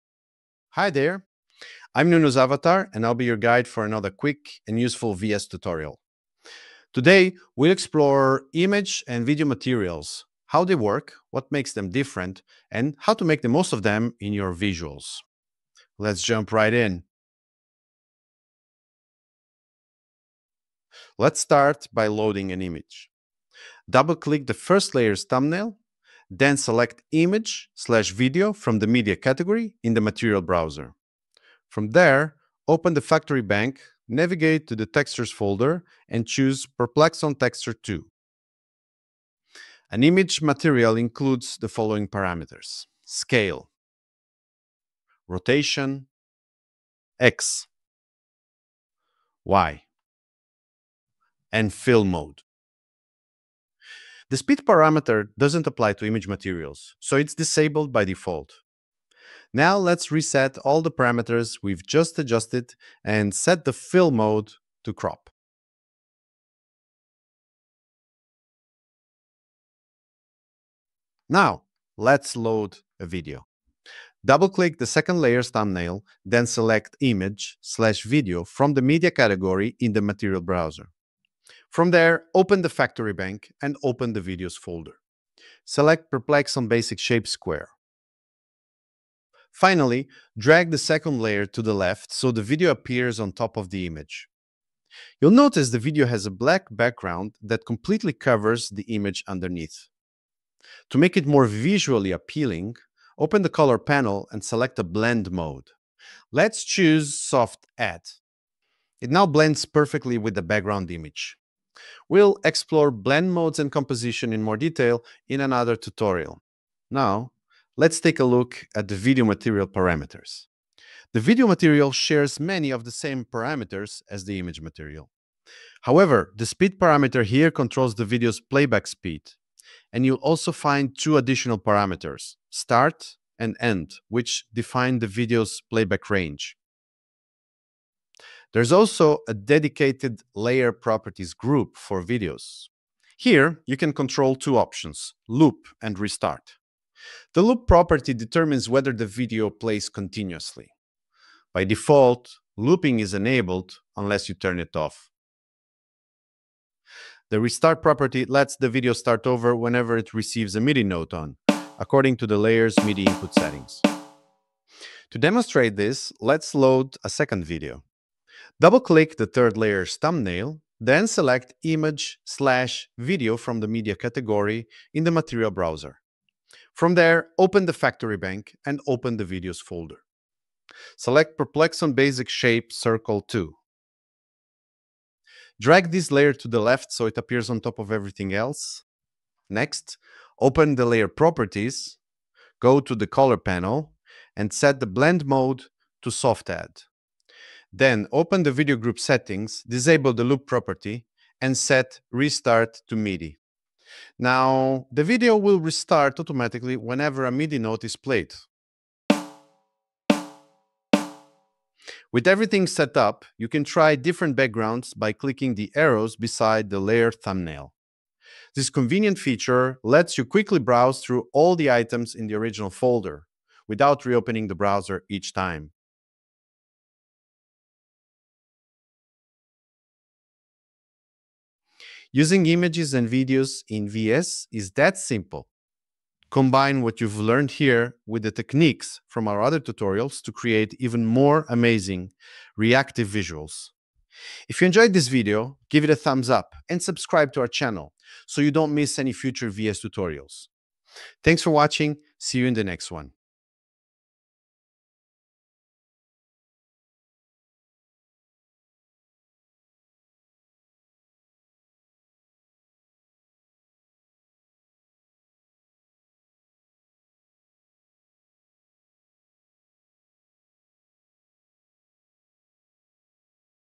VS 2 Tutorial - Images & Videos
In this quick and practical tutorial, Nuno’s avatar guides you through everything you need to know about image and video materials in VS — from loading and adjusting them, to blending layers and syncing video playback with MIDI.

You’ll learn how to:

✅ Load image and video materials from the Factory Bank
✅ Adjust key parameters like scale, rotation, and fill mode
✅ Blend visuals using Soft Add and other blend modes
✅ Control video playback with Loop and Restart properties
✅ Trigger videos with MIDI notes for dynamic, reactive visuals

📌 Chapters:

0:00 Intro
0:17 Images
1:07 Videos
3:47 Wrap up